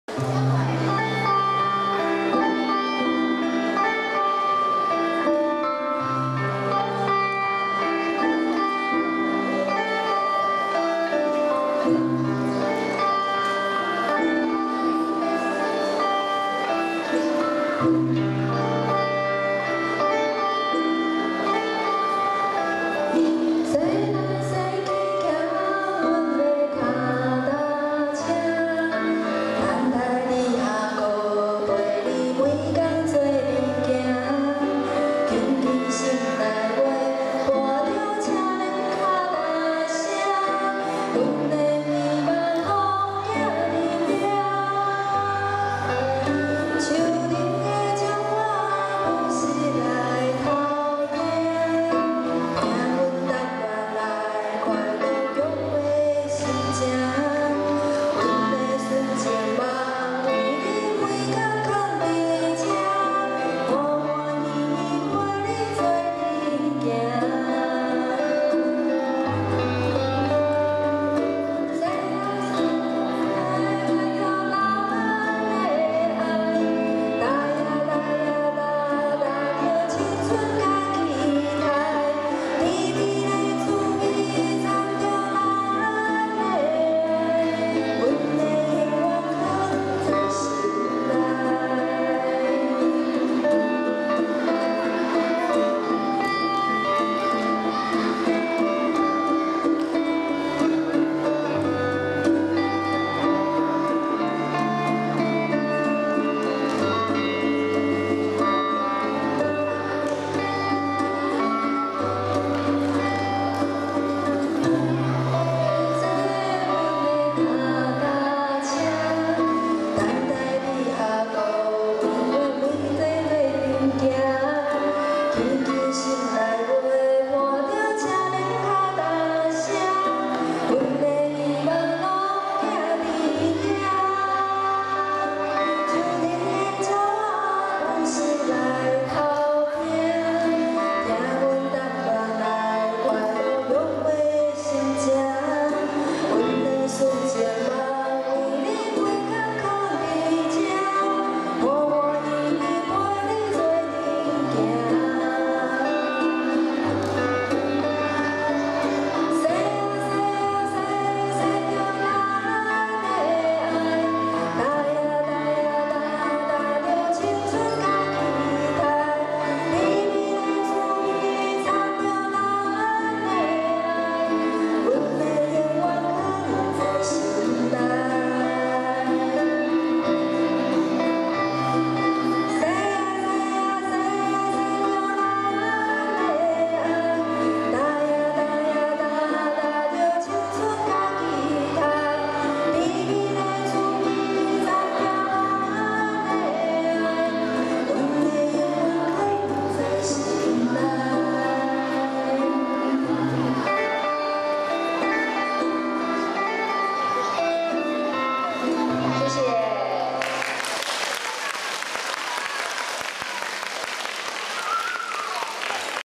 虎山國小2016才藝發表會 三乙 簡聿宸 、陳孟婷 歌唱演奏／腳踏車
 時間：105年5月17日8:30～10:00時
 地點：虎山國小活動中心